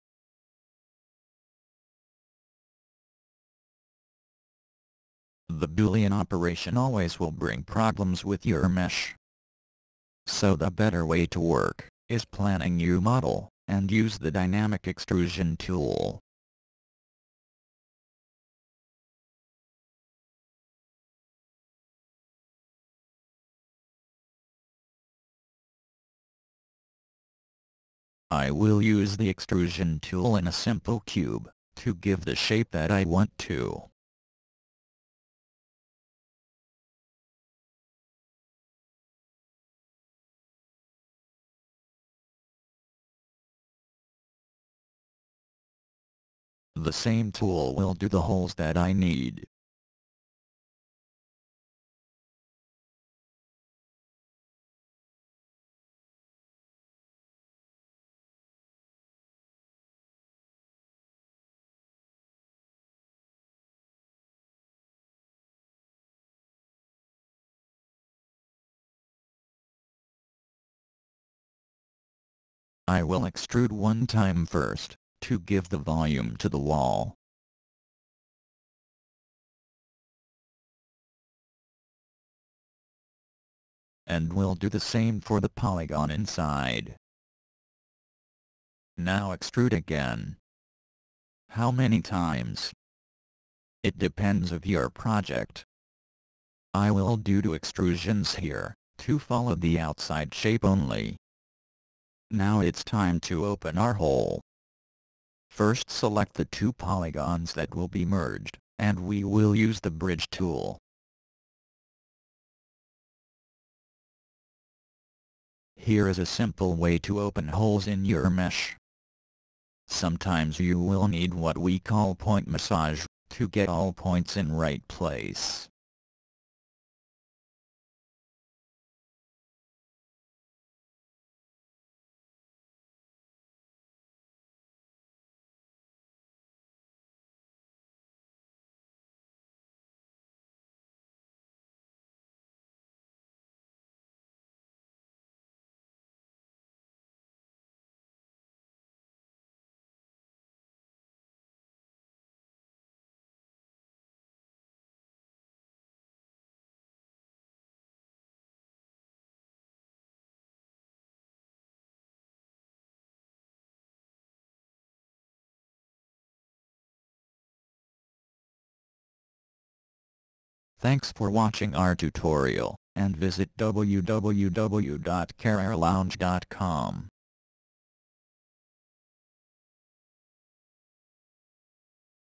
Hole no boolean
A simple way to create hole sin objects without using boolean